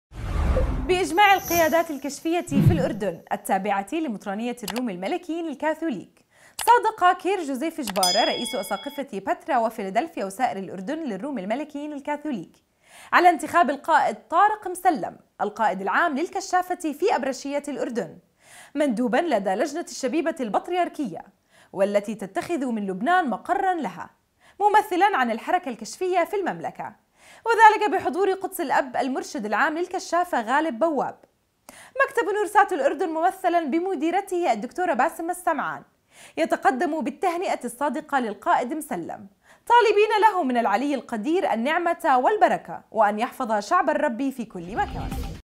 نورسات الأردن : القائدُ الكشفي طارق مسلّم مندوباً للجنةِ الشبيبةِ البطريركية
بإجماع القيادات الكشفيةِ في الاردن / التابعةِ لمطرانيةِ الروم الملكيين الكاثوليك /  صادق كير جوزيف جباره رئيسُ اساقفةِ بترا وفيلادلفيا وسائرِ الاردن للروم الملكيين الكاثوليك / على انتخابِ القائد طارق مسلم القائدِ العام للكشافةِ في أبرشيةِ الاردن / مندوباً لدى لجنةِ الشبيبةِ البطريركيه والتي تتخذُ من لبنانَ مقراً لها / ممثِلاً عن الحركةِ الكشفيةِ في المملكة / وذلك بحضور قدسِ الاب المرشد العام للكشافه غالب بواب / / مكتبُ نورسات الاردن ممثلا بمديرته الدكتورة باسمه السمعان / يتقدم بالتهنئةِ الصادقة للقائد مسلم / طالبينَ له من العليِ القدير النعمةَ والبركة / وأن يحفظَ شعبَ الربِ في كلِ مكان .